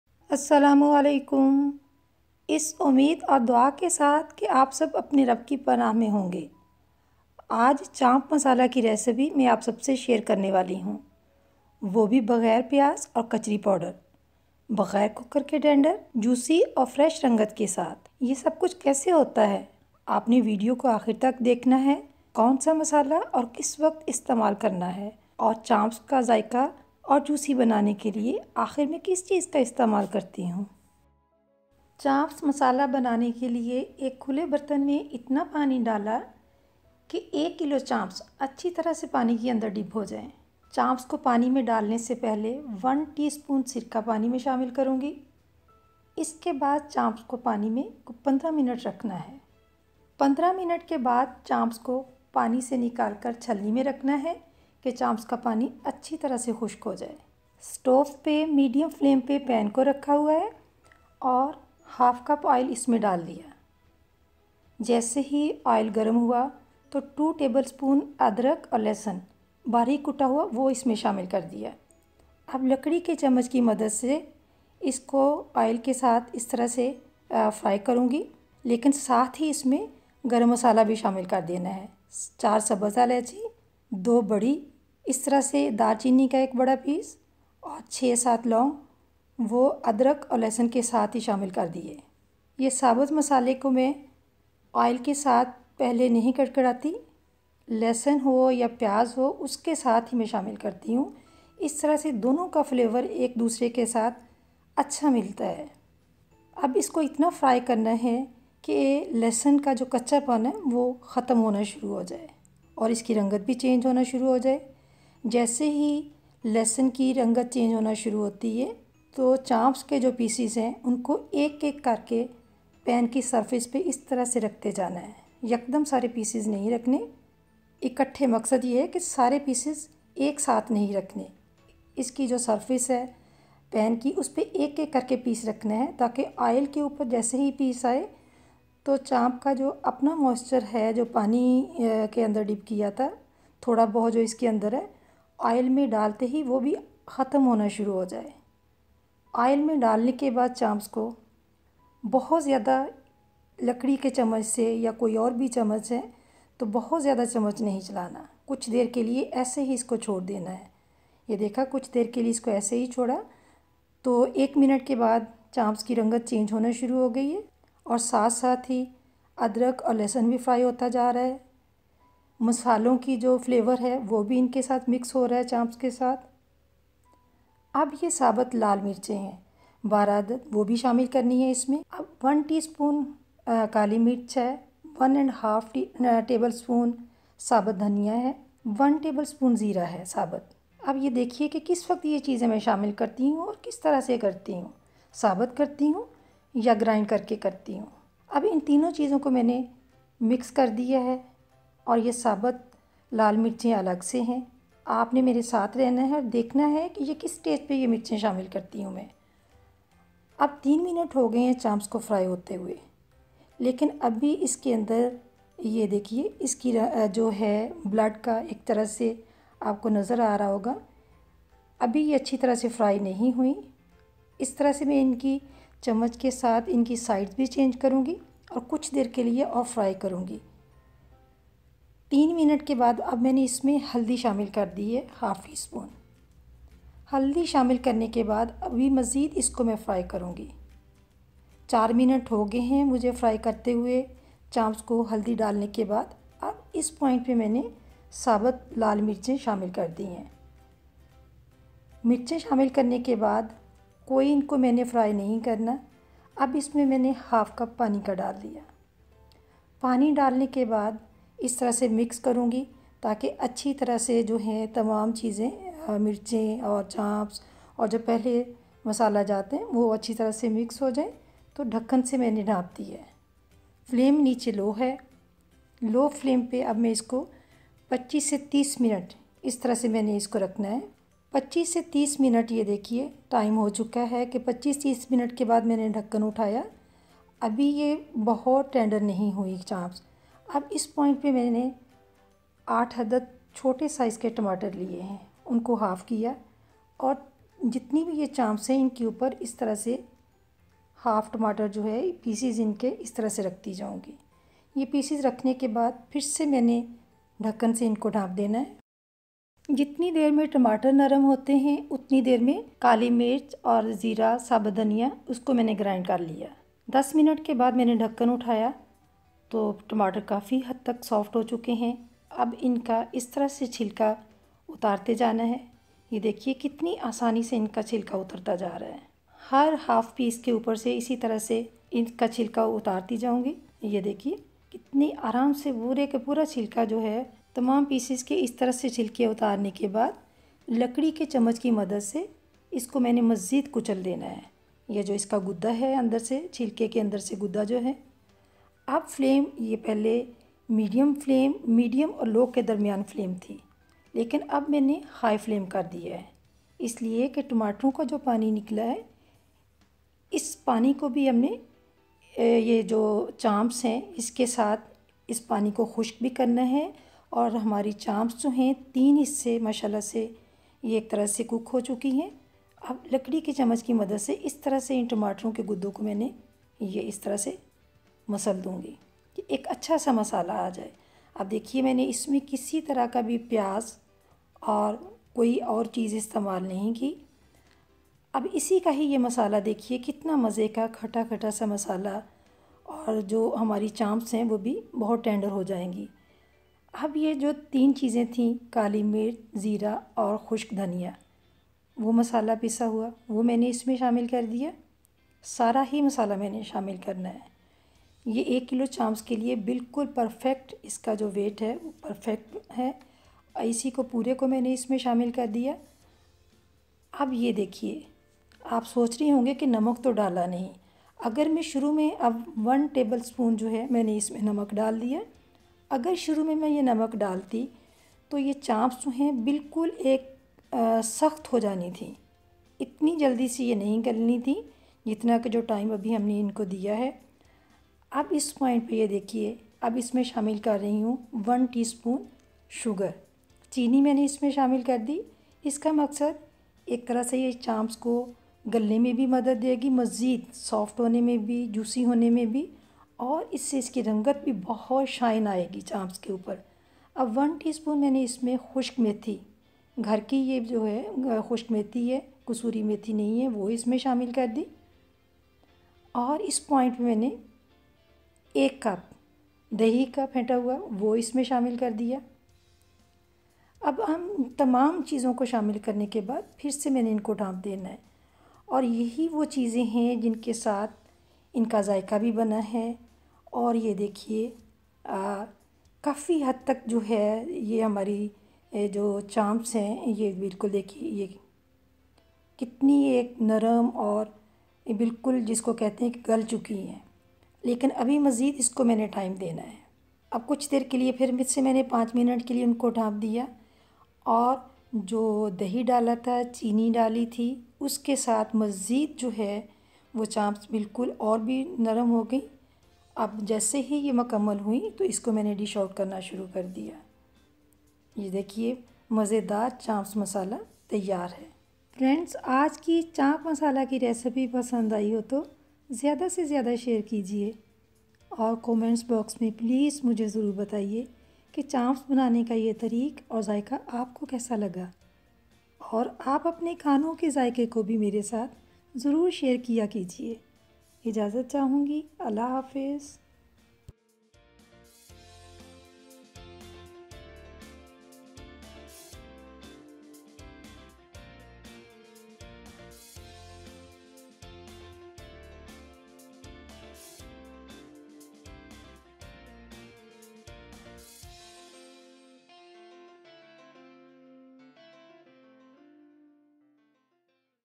Masalaydar Mutton Chops Recipe without Oven / Cooker | Bhuna Gosht Chaap Masala (Soft and Tender)
In this video I will share with you my recipe on how to make masala mutton chops recipes without using any oven or cooker. Make tender and juicy mutton masala chops.

Ingredients

(first soak the chops in pot full of water along with 1 tbsp of vinegar for 15 minutes)

1 cup oil,
2 tbsp ginger garlic paste,
4 green cardamom,
7 cloves,
1 cinnamon stick,
2 black cardamom,
12 whole red chilies,
1 tsp whole black pepper,
1/2 tsp whole dry coriander,
1 tsp whole cumin,
1/2 tsp turmeric powder,
8 whole tomatoes,
1 tbsp salt,
1 tsp sugar,
1 tsp funnel seeds,
1 cup yogurt,
4 fresh green chilies,

Method;

1. In a pan add oil then add ginger garlic paste along with green, black cardamom, cinnamon and gloves and fry for 2 to 3 minutes until fragrant.

2. Now add chops and fry them for 10 minutes then add turmeric powder along with red chilies and a cup of water and cook it on low heat with a lid on for 15 minuter.

3. Now add tomatoes and cover it again and cook it for 20 to 25 minutes then remaining powdered spices along with sugar and yogurt and cook it for 15 more minutes then add green chilies and serve it with naan and enjoy.

Recipe Instructions

0:00 Masalaydar Mutton Chops Intro
0:32 Add Chops to Vinegar Water
1:05 Cooking Instructions
8:37 Final Presentation Mutton Chops